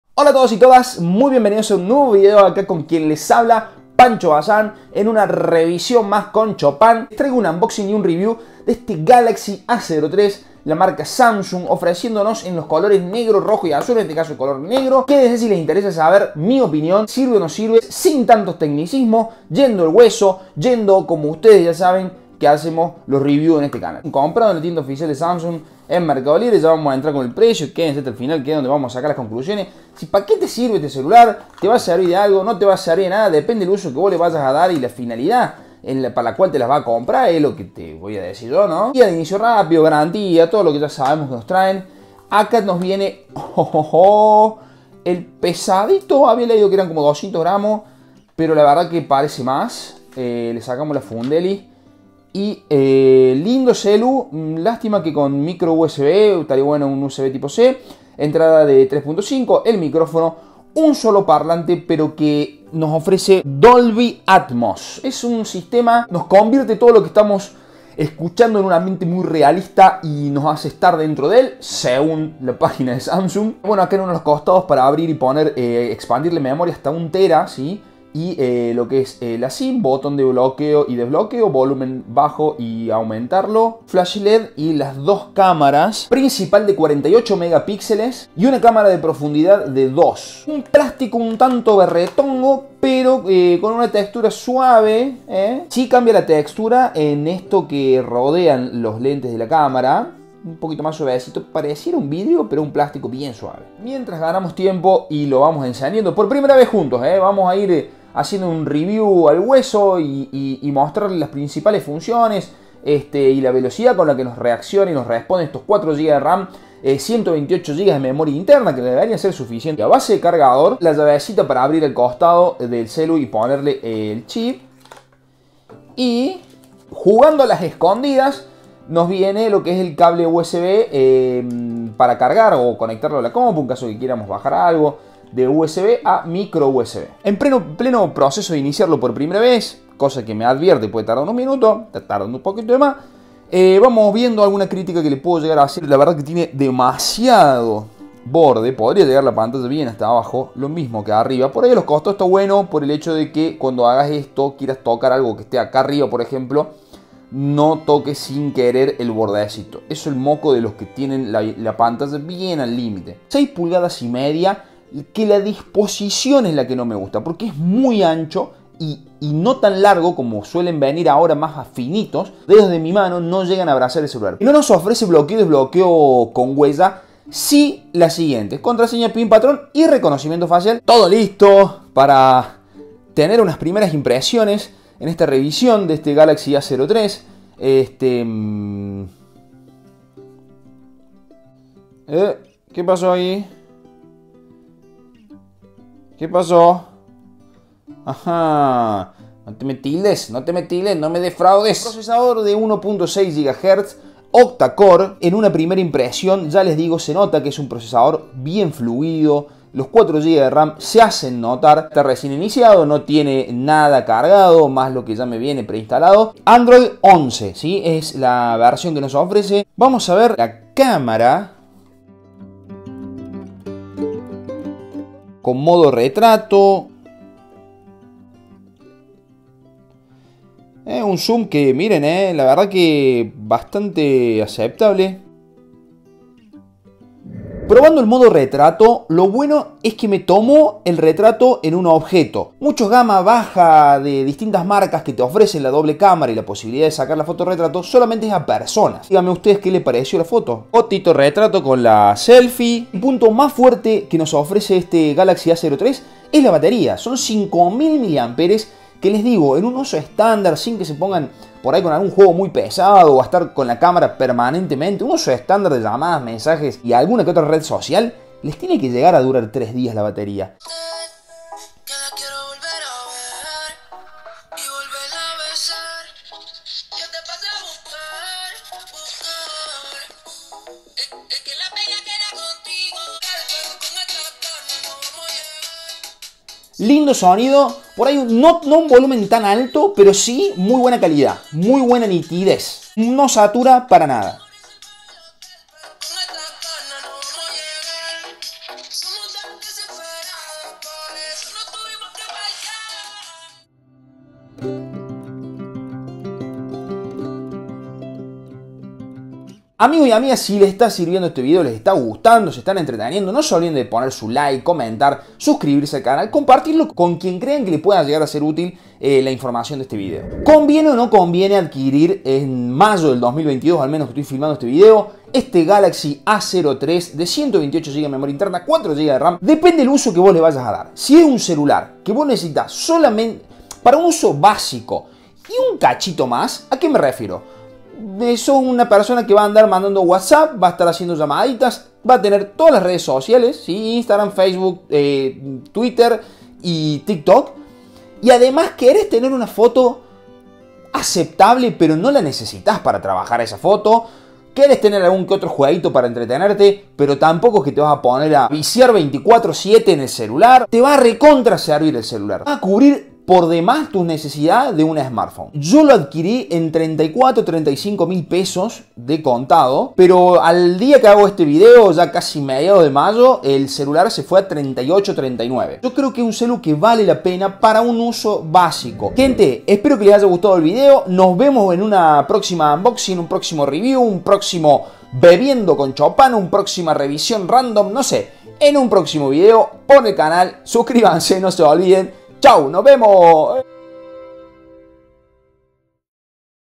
Samsung galaxy a03 reviewenespañol 128gb 4gb 📲
Buenas ! Cómo andan? Aquí les dejo mi opinión sobre el Samsung galaxy a03 , en este caso de 128 gb de memoria interna y 4gb de ram !
Tengan en cuenta , que todos los a03 galaxy, tanto de 32, 64 y 128 gb son el mismo mismo tél , solo cambia la memoria

Espero les guste y la disfruten y se entretengan por sobre todas las cosas jeje !

Mis redes: panchobazan
Produ: facuabudr